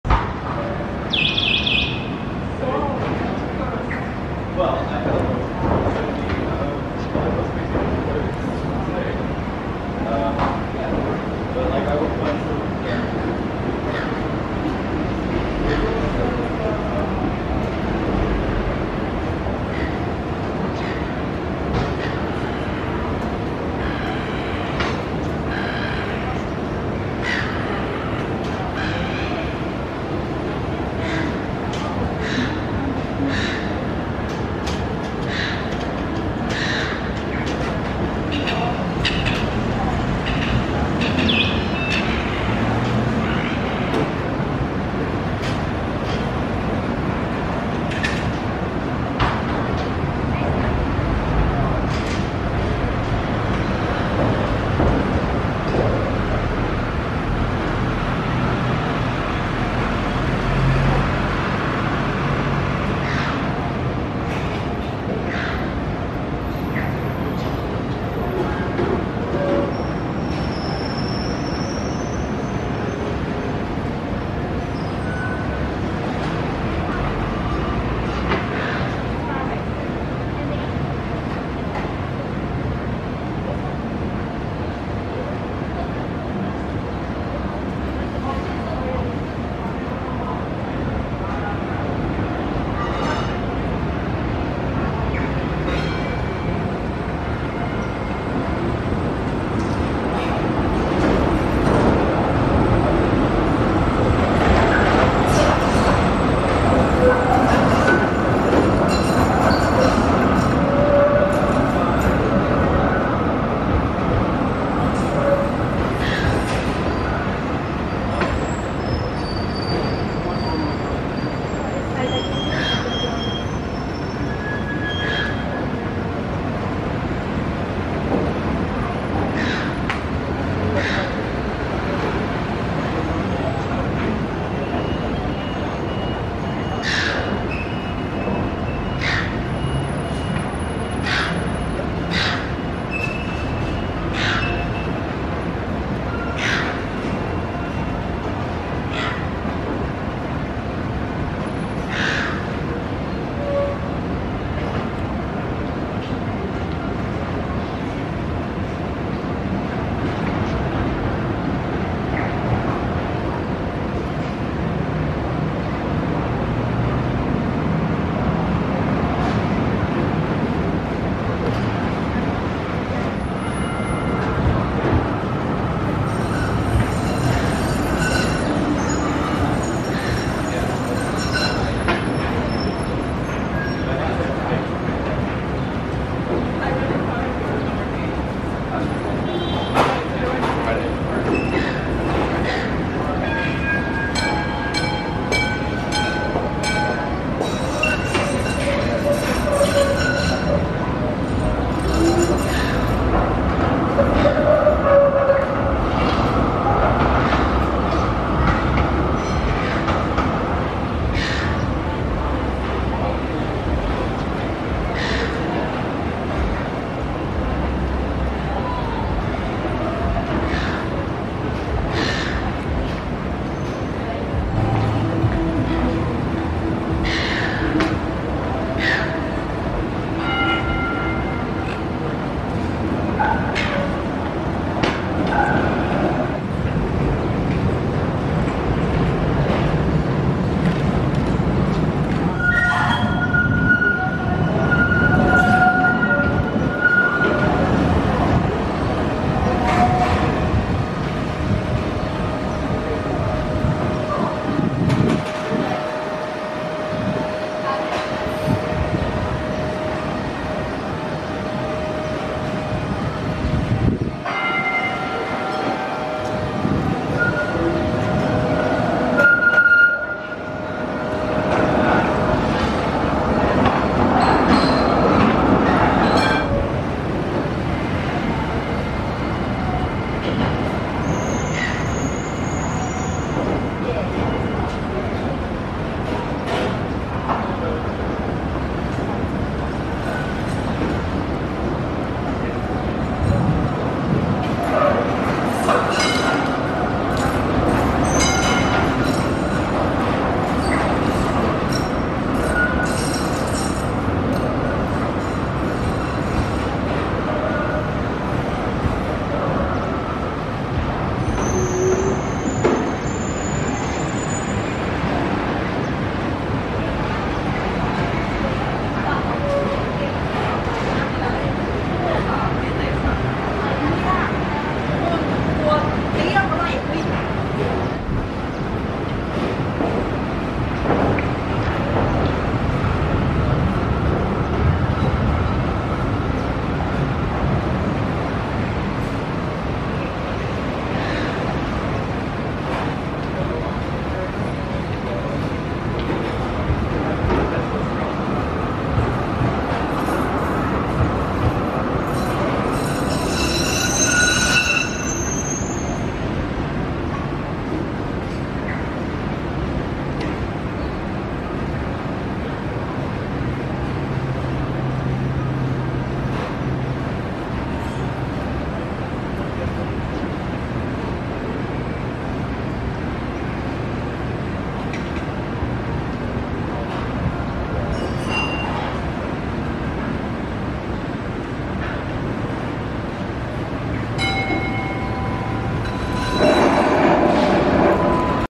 Pigeons eating a piece of a chocolate biscuit
There were two birds here happily pecking away at a sweet snack on the city streets and they pretty much had it all to themselves. Although there was one seagull, it didn't eat much and left the scene. They couldn't seem to get enough of this food so they must have really enjoyed it.